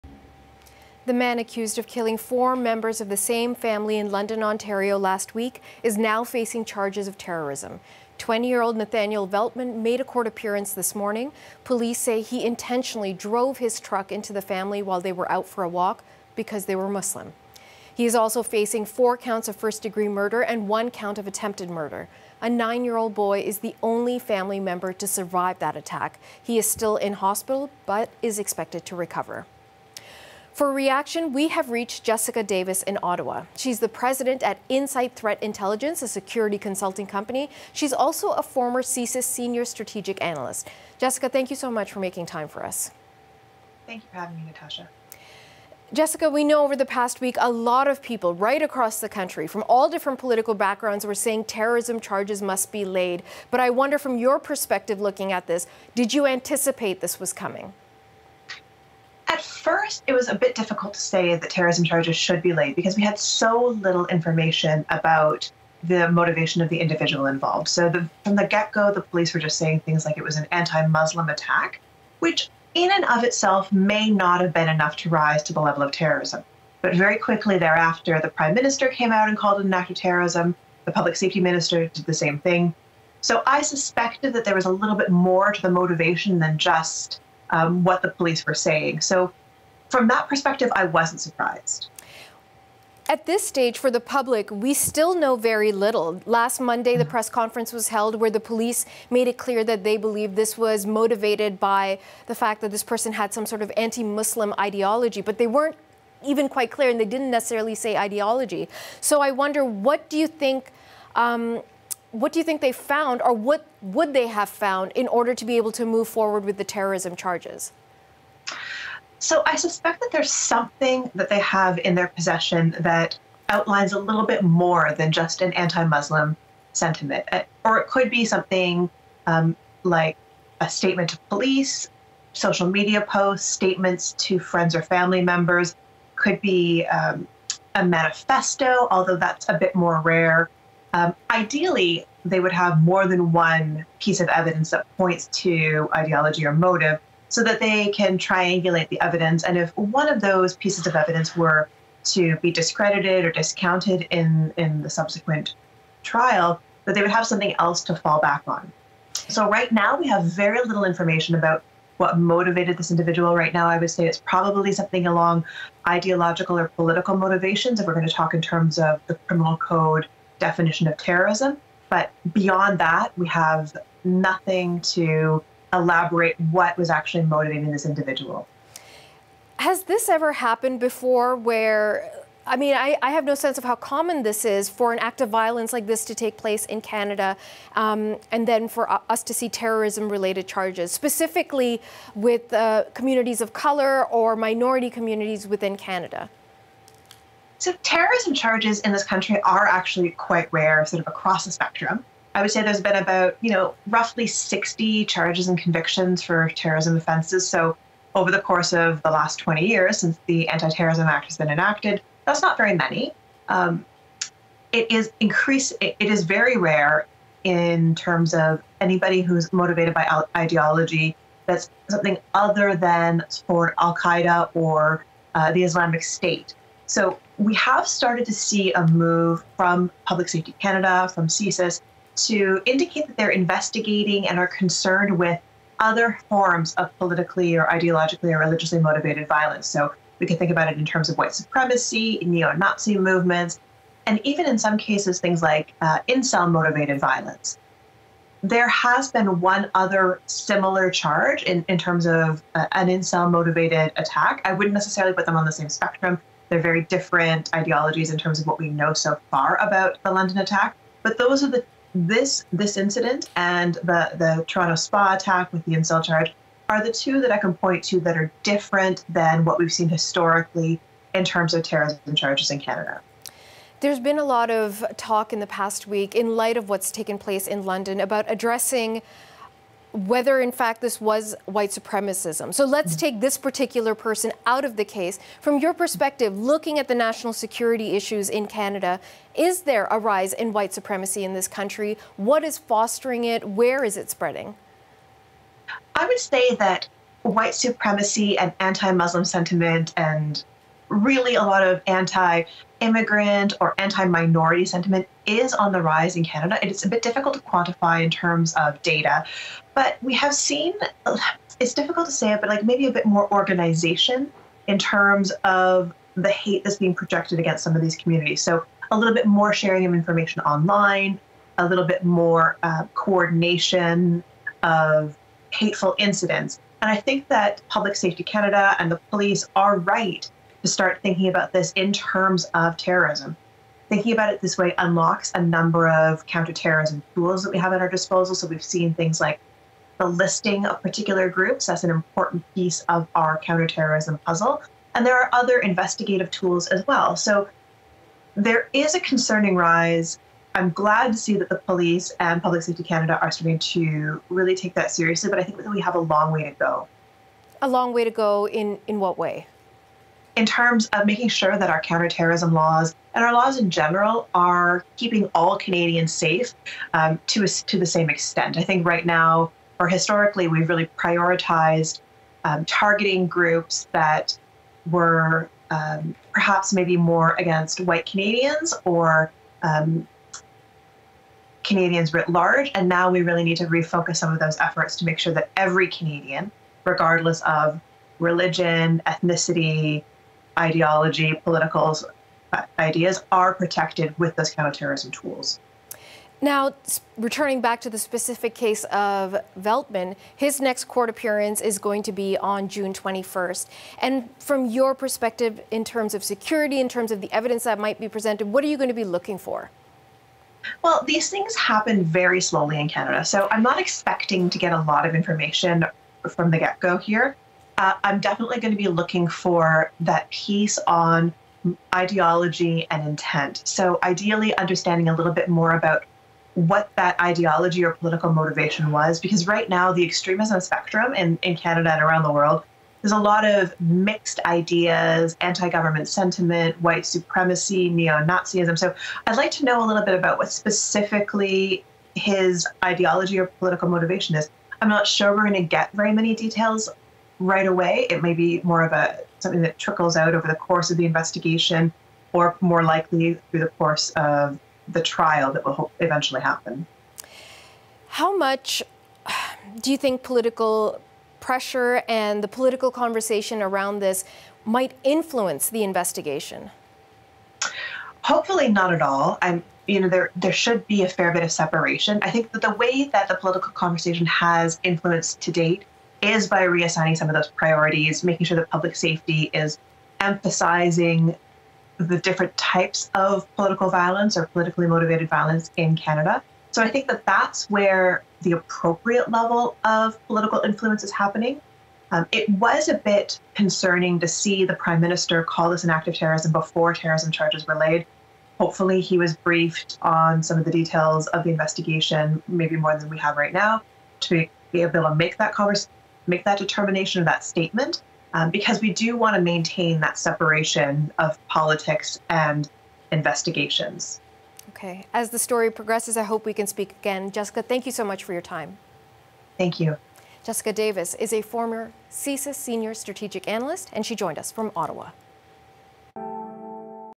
Accused in London, Ont., vehicle attack now facing terrorism charges
Nathaniel Veltman has been charged with two terrorism-related offences in connection with the fatal hit and run that left four members of a Muslim family in London, Ont., dead and a nine-year-old boy in hospital. The new charges come a week after he was charged with four counts of first-degree murder and one count of attempted murder.